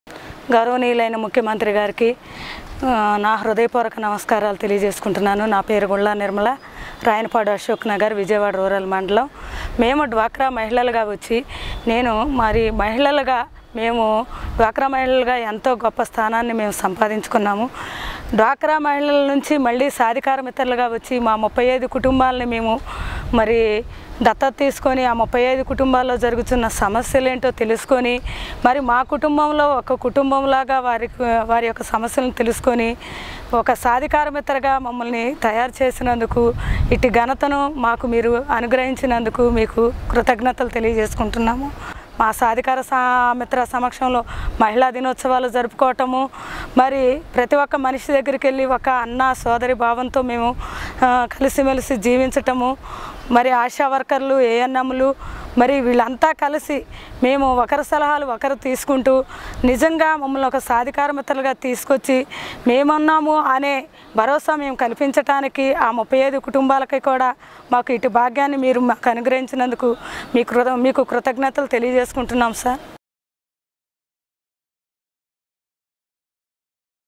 Empowering Women through DWCRA & Sadhikara Mithra | Krishna District
The video shows Mrs Nirmala explained how she is contributing to empowering women in the State through DWCRA and Sadhikara Mithra.